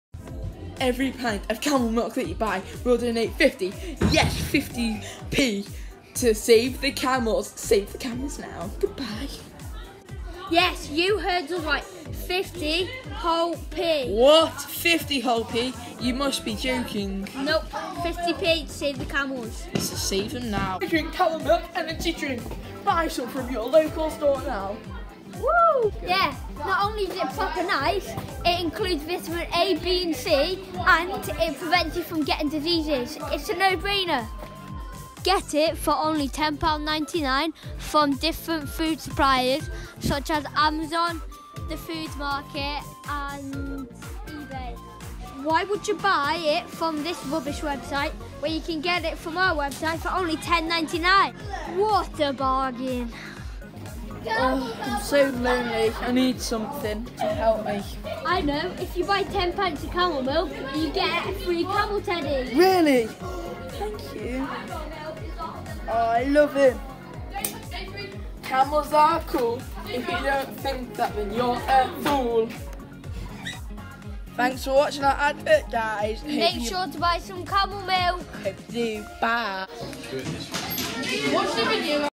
Camel Milk 2
Year 8 have been making adverts for camel milk.
for more updates.